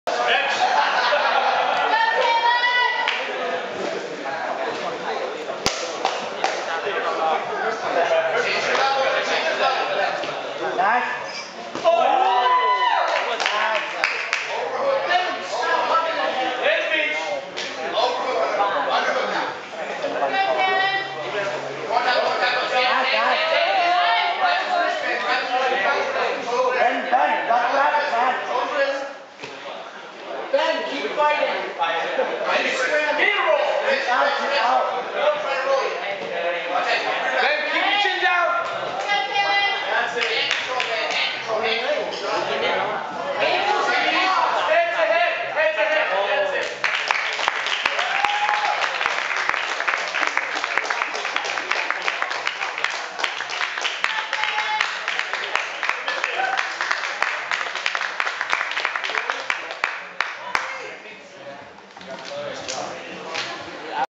2011 No-Gi Festival Beginner 73.6-79.6 kg: Taylan Ozmalkoc VS Ben Dixon
2011 No-Gi Festival Beginner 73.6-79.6 kg: Taylan Ozmalkoc VS Ben Dixon both from BKKBJJ